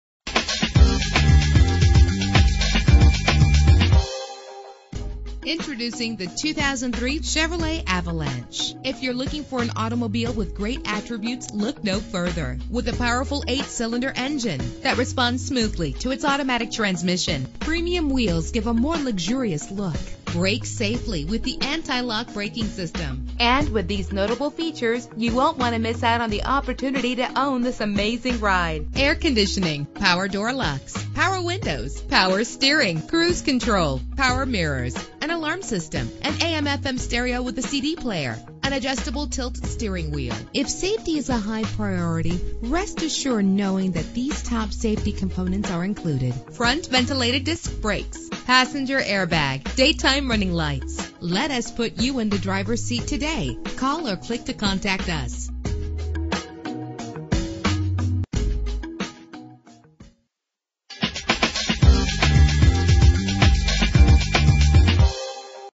2003 Chevrolet Avalanche - Springfield OH
Year: 2003
Make: Chevrolet
Model: Avalanche
Trim: 1500
Engine: 5.3 liter 8 cylinder 16 valve
Transmission: 4-Speed Automatic
Color: Orange
Mileage: 91814
Address:
1501 Hillcrest Ave